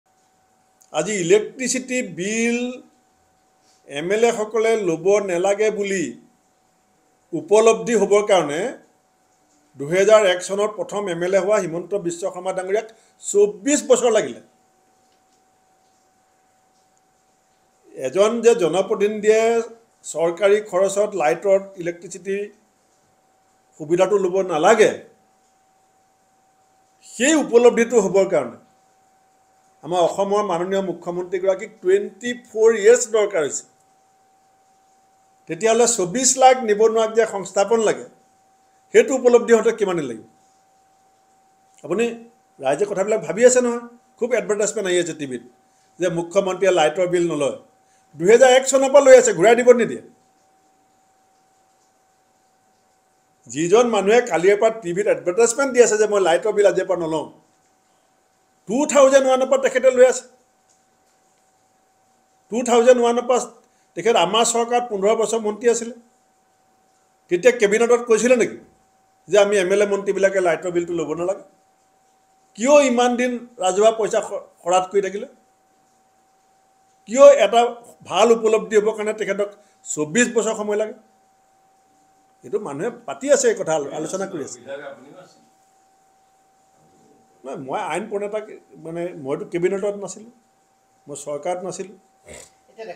Bhupen Borah on CM Himanta | চৰকাৰী বিদ্যুৎ বিল পৰিশোধ সন্দৰ্ভত ভূপেন বৰাই উভতি ধৰিলে CM ক | N18V
Bhupen Borah on CM Himanta | চৰকাৰী বিদ্যুৎ বিল পৰিশোধ সন্দৰ্ভত ভূপেন বৰাই উভতি ধৰিলে CM ক |